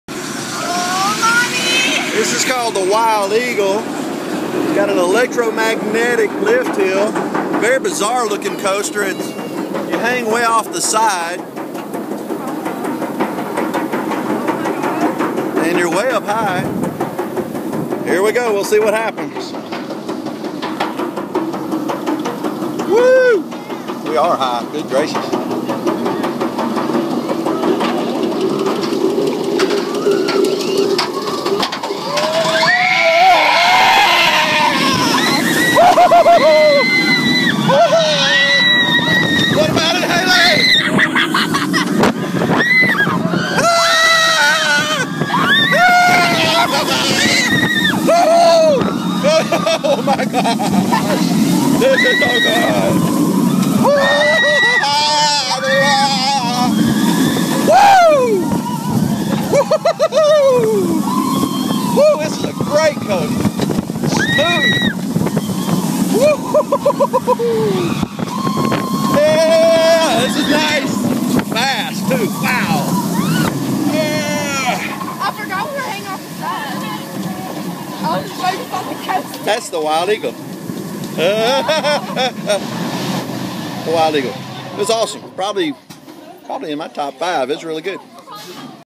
Scott rides a roller coaster part 2: The Wild Eagle
Let's ride America's first wing coaster, The Wild Eagle, together. Awesome!